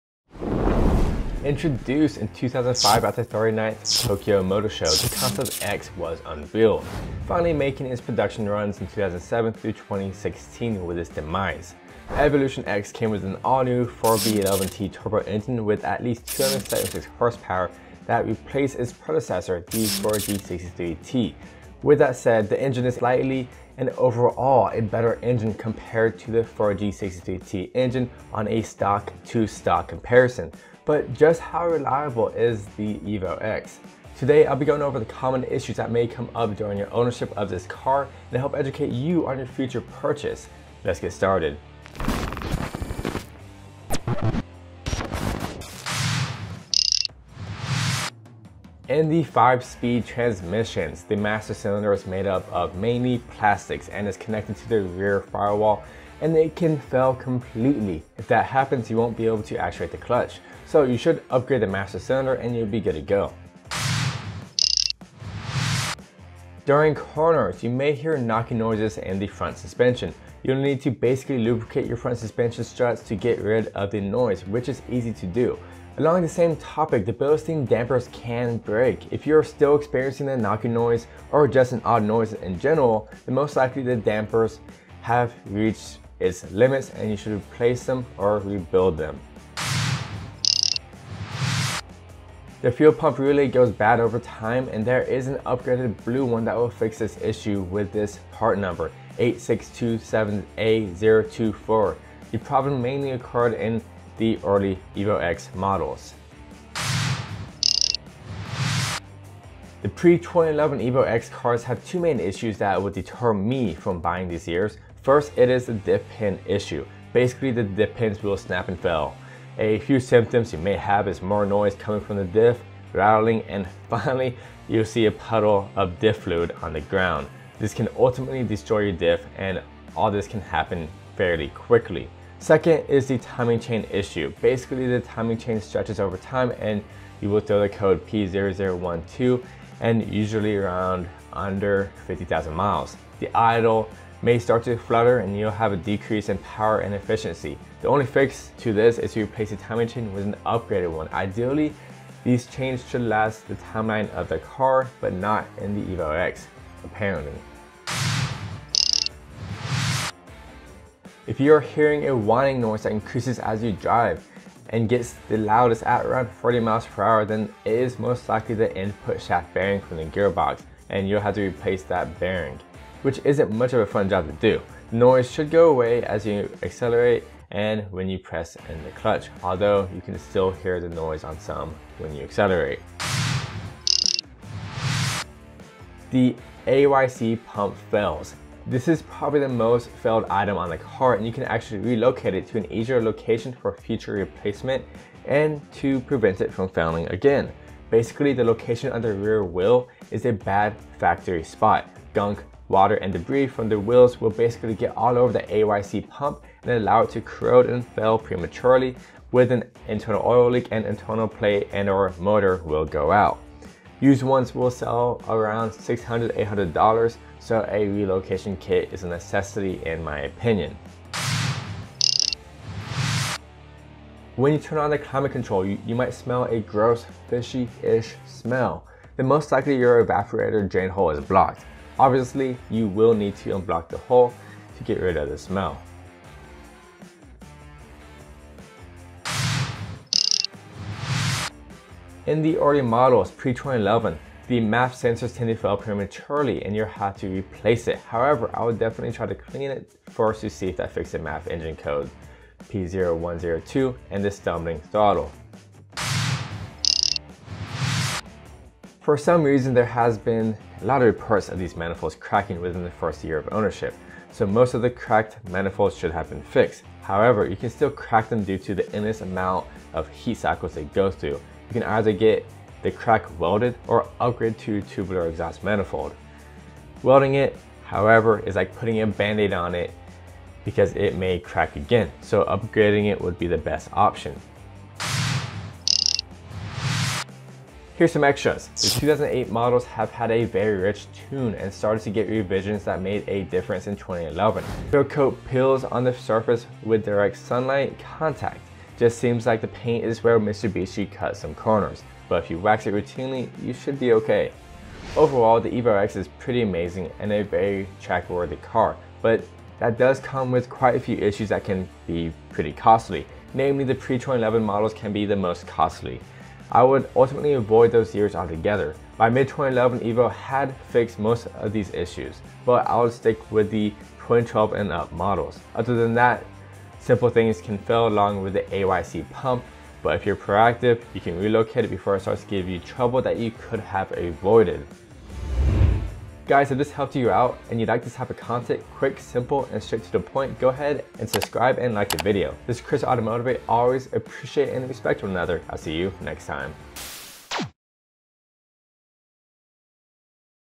Watch This Before Buying a Mitsubishi Evo Lancer 2008-2015
If you have or wanting to buy a Mitsubishi Evo X, then watch this first! I break down the common issues and reliability to help you diagnose certain issues you may have or better educate you on your future purchase.
The Evolution X is a great car however, it does have its fair share of issues.

Youtube.com/Automotivate

Have questions or just want to chat?